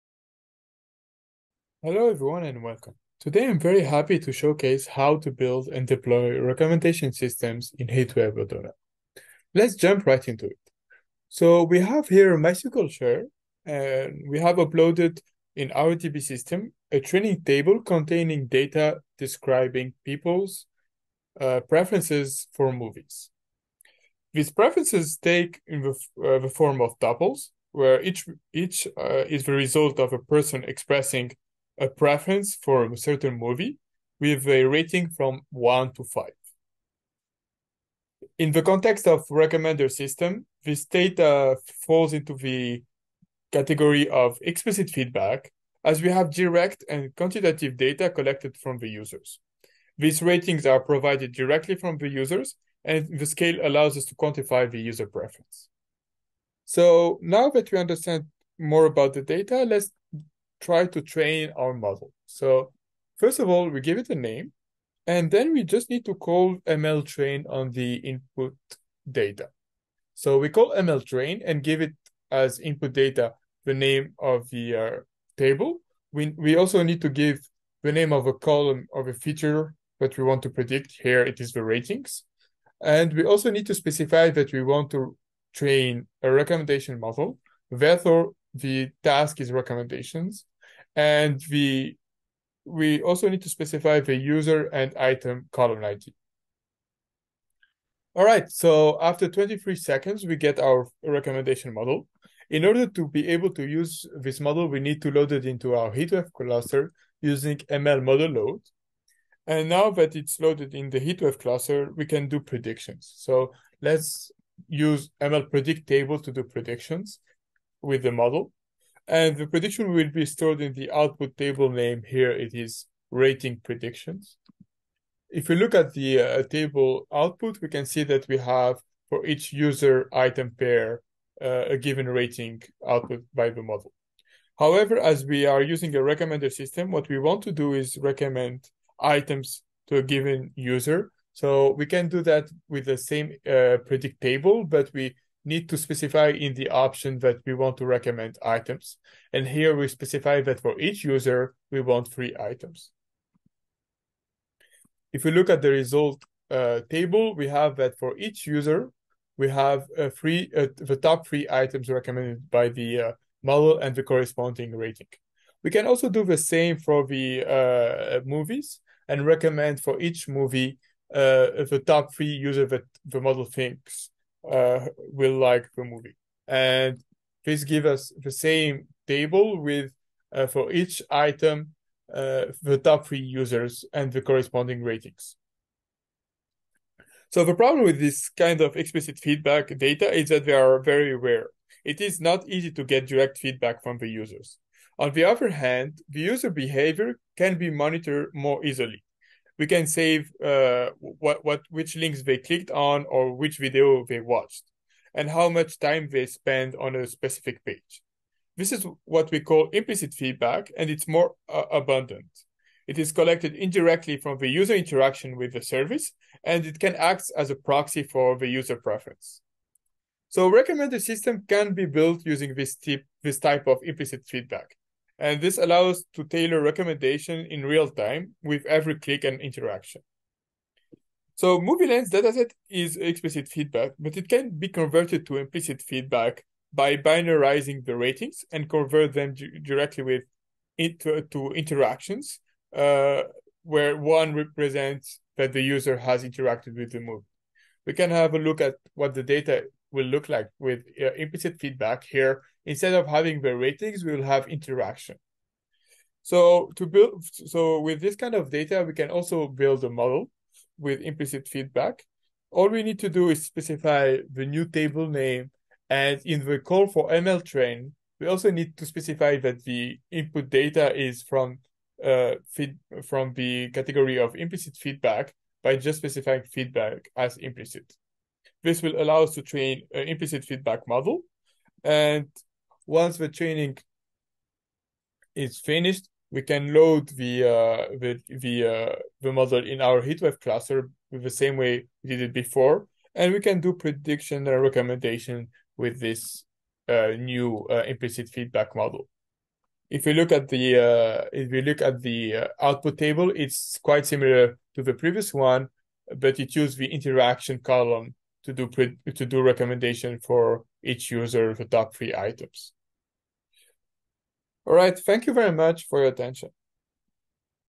Demo: Recommender system with MySQL HeatWave AutoML
In this video you will learn how train a recommender system model using HeatWave AutoML and make predictions for the data stored in HeatWave. It shows the building of the model using both explicit and implicit feedback.  HeatWave AutoML automates various steps of the machine learning, thus saving customers significant time and effort. Data does not leave the database, and customers do not have to install, configure, and manage multiple services, saving time, money, resources, and compliance overheads.